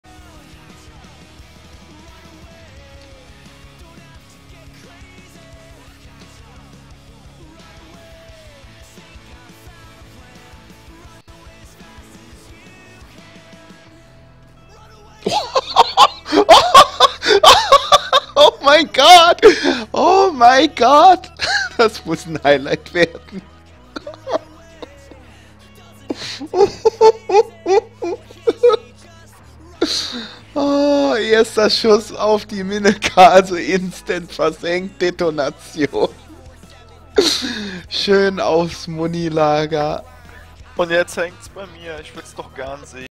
Kohlberg vs Minekaze oneshot *pewpew*
Was playin with my Kohlberg when a Minekaze came on me behind an island and i hit it which ended in a detonation.